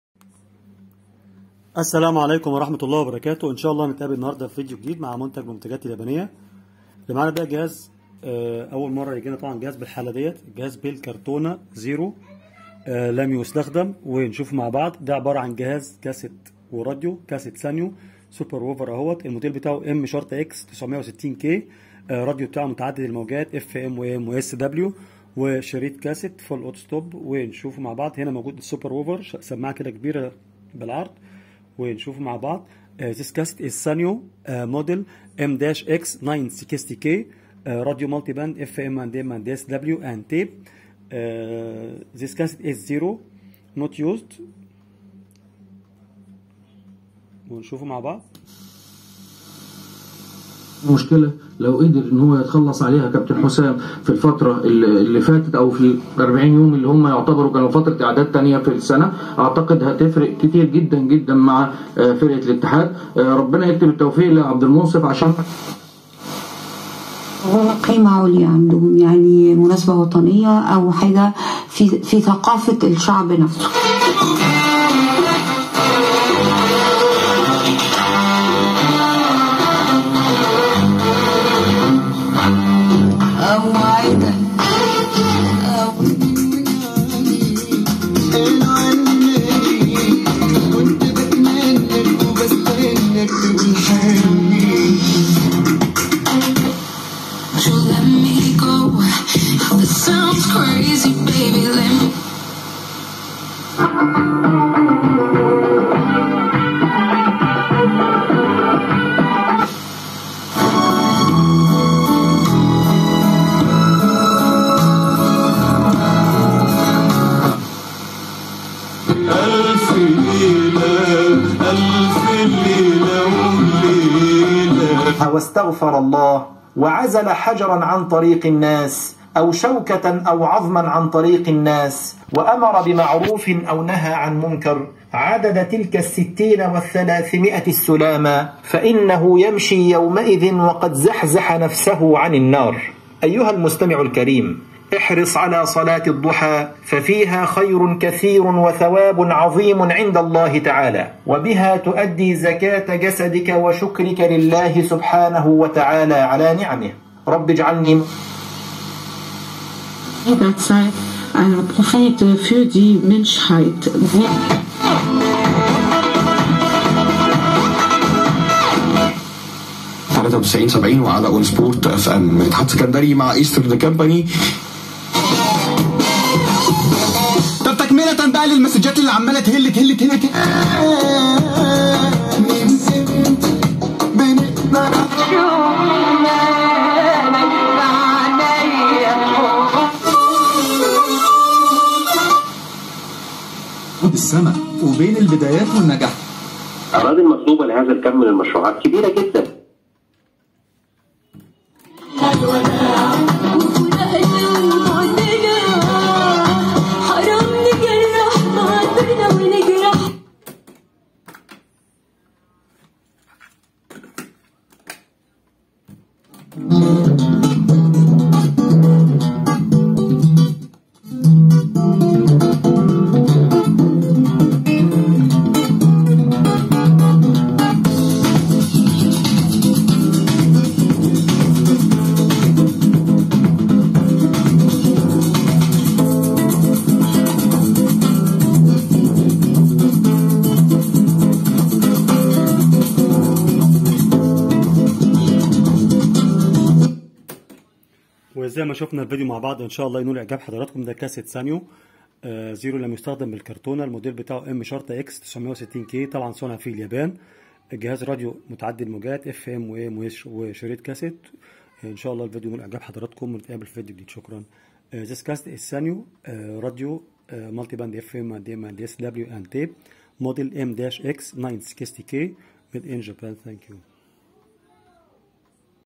كاسيت سانيو موديل M- X960K + راديو FM - AM - Sanyo M-X960K Audio Cassette Player + FM Radio - AM
كاسيت سانيو موديل M- X960K +  راديو  FM  - AM
صنع فى اليابان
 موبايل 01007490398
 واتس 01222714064
وما تنساش تشترك فى القناه ليصلك كل جديد

Sanyo M-X960K Audio Cassette Player + FM Radio - AM
made in Japan
 To communicate